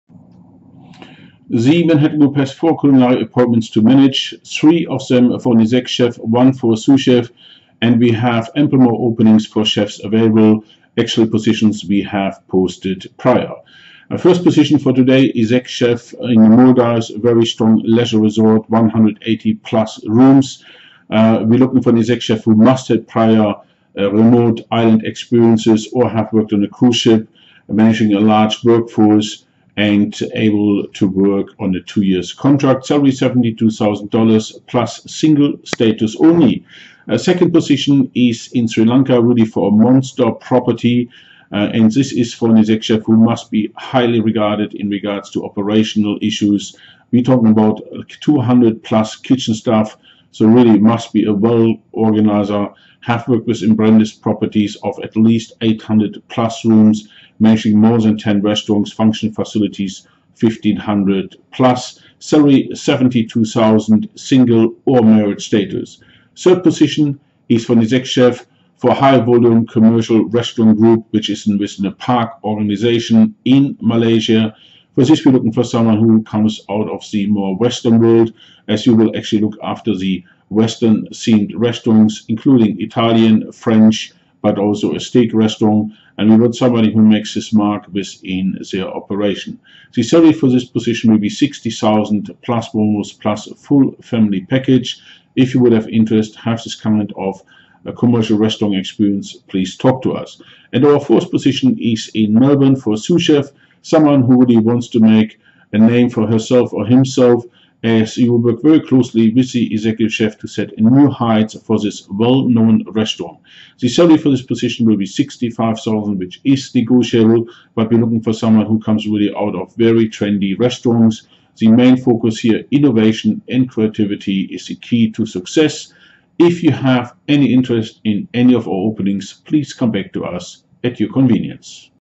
4 Executive & Souschefs- Maldives- Australia- Sri Lanka & Malaysia
Culinary Appointments
4 Executive & Souschefs
Maldives- Australia- Sri Lanka & Malaysia

Executive Chef- 8 F&B Outlets for 180 Room Resort- Maldives- US66-72
Must have island or Cruise ship experience
Confident within large ops managing buffet & specialty restaurants
Single status only

Executive Chef- 200+ Kitchen Staff within Hugs Ops- US$72- Sri Lanka
Huge kitchen brigade with 200+ staff
Directing large catering facilities & 12 F&B outlets
Single or married status only

Executive Chef- High Volume Commercial Restaurant Outlets within Park Operation- US60+ Bonus+ Family Package
Be part of an integrated & premium resort location
Special emphasis on western food within high volume commercial restaurant output
Drive consistency through training & educating the local workforce

 Souschef- Work with New Appointed Executive Chef in Semi Fine Dining- Melbourne- Western Suburbs- $65
Work closely in menu design & promotion with New appointed Exe chef
Establish the best possible product for lunch & dinner
Innovation & creativity is the key to success
Immense opportunities within a blue-chip company